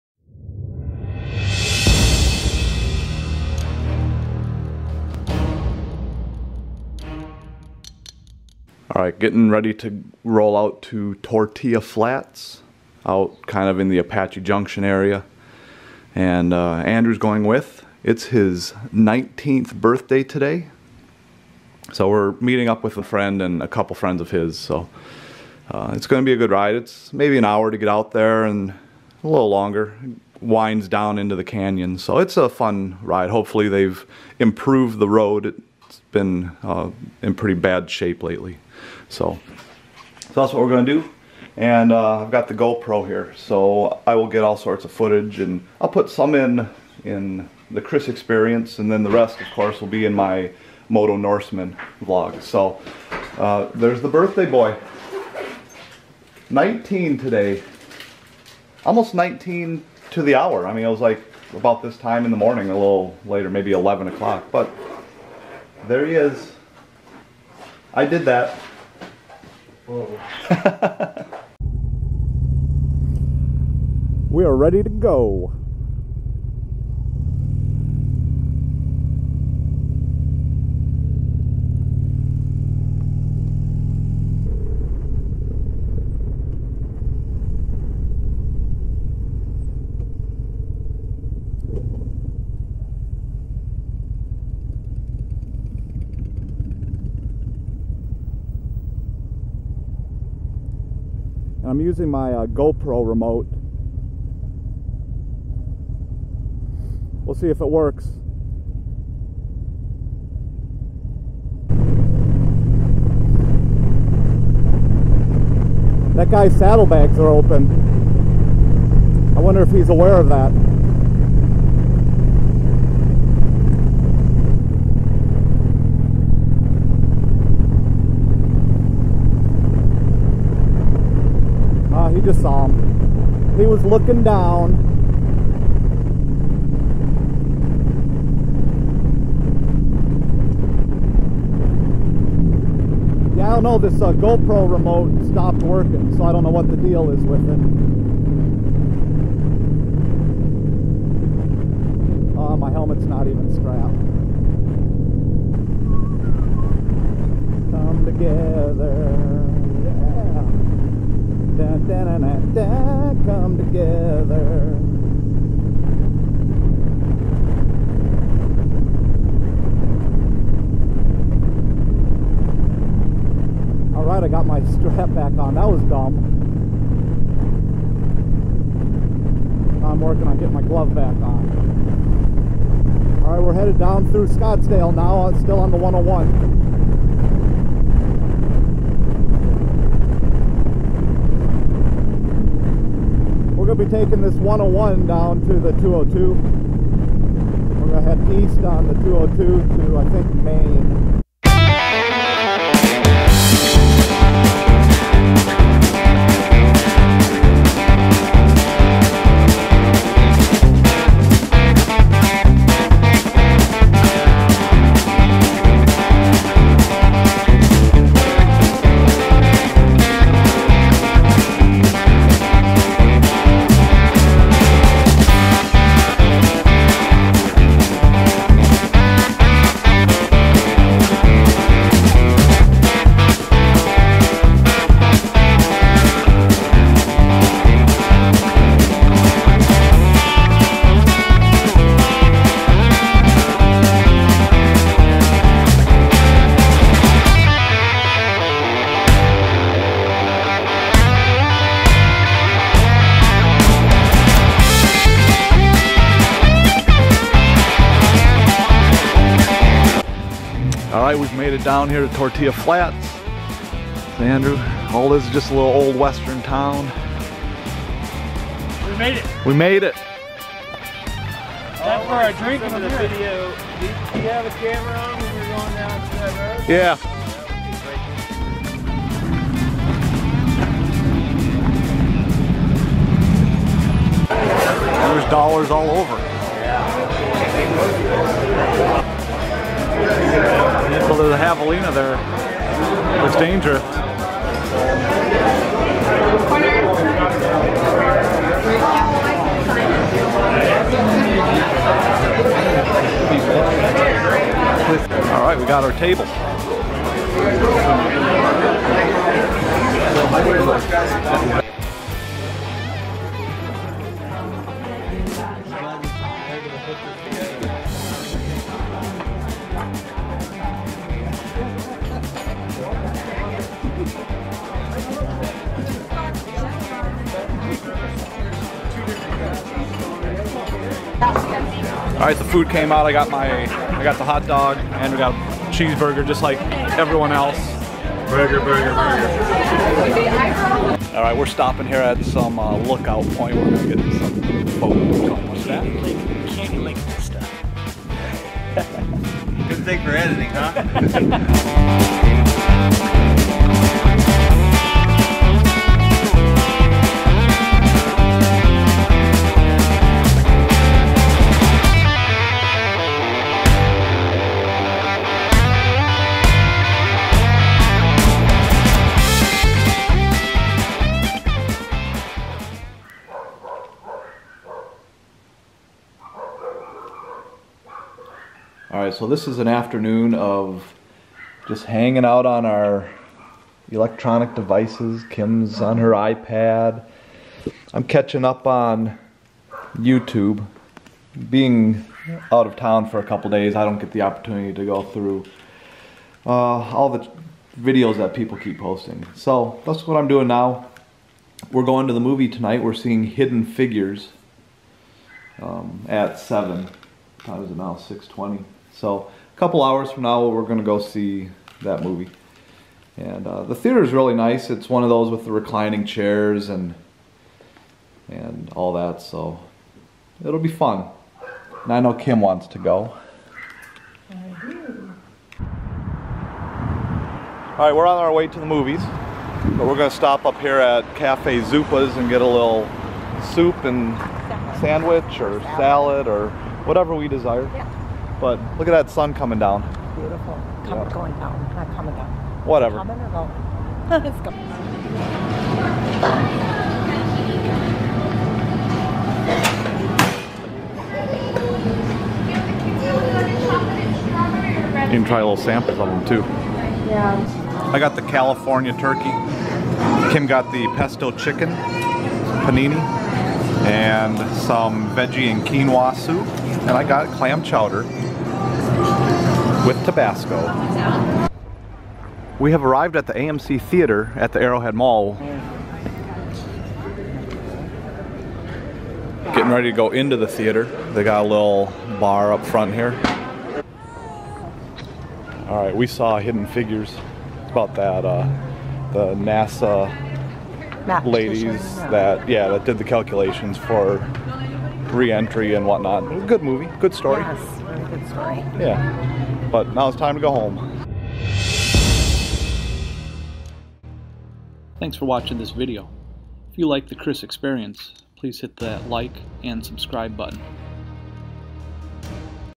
Tortilla Flat & Hidden Figures! | 02-25-17
Andrew and I ride down to Tortilla Flat in the morning and then Kim and I go to the movies in the evening!  It's a jam packed day!  Check it out!